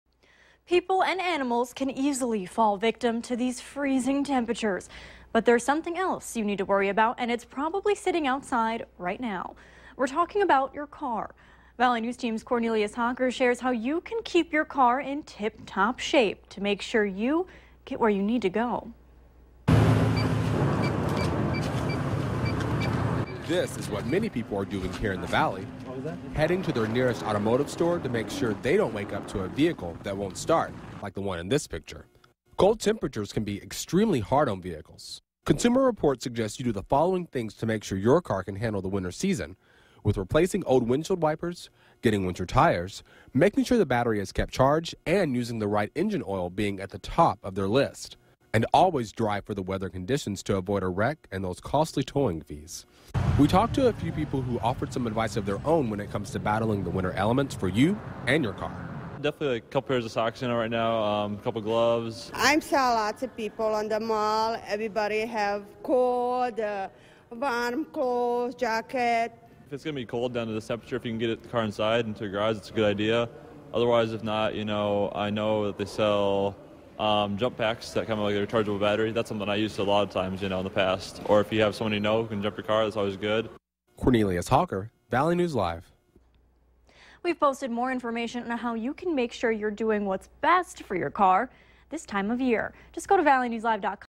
Winterizing your car
Valley New Team's Cornelius Hocker shares a  few ways you can keep your car in tip top shape this winter season.